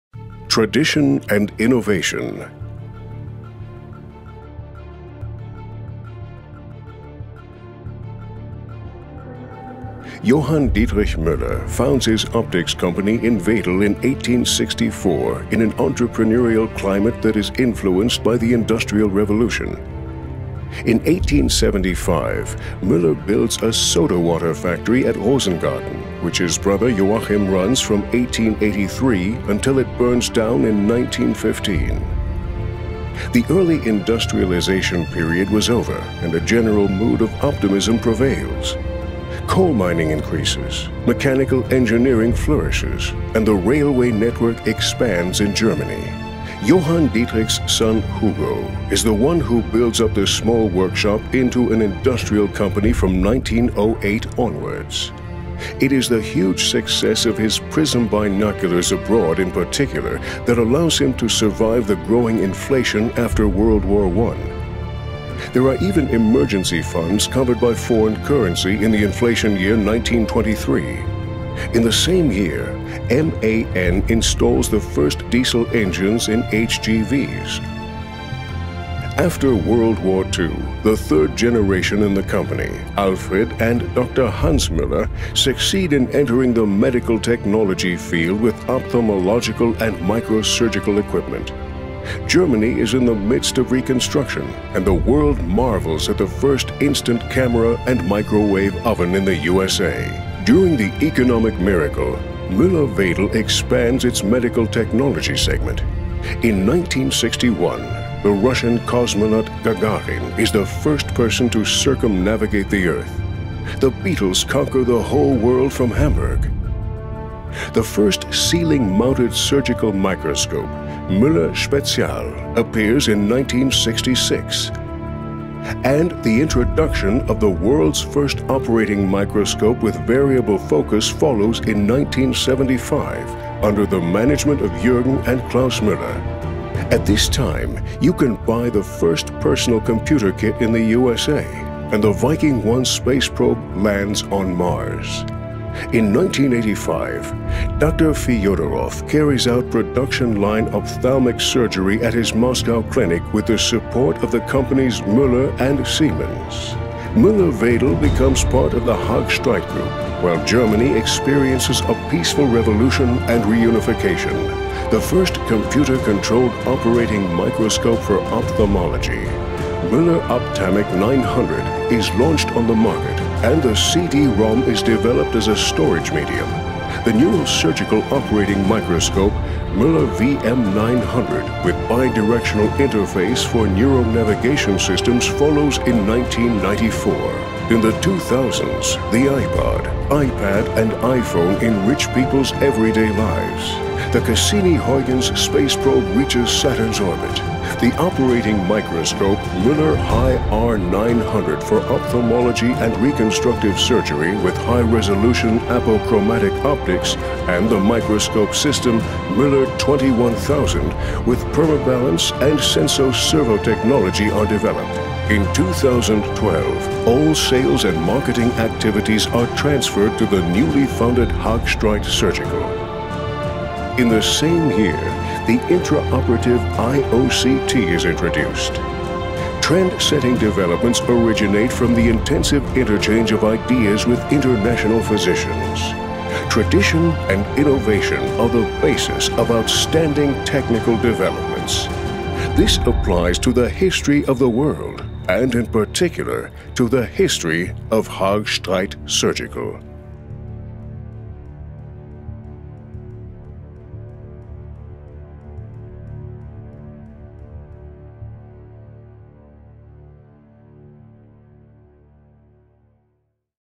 A History of Excellence in Surgical
Haag-Streit has gone through a number of changes since its roots in Wedel, Germany in 1864. What hasn't changed is our commitment to quality and innovation. Learn more about our great history.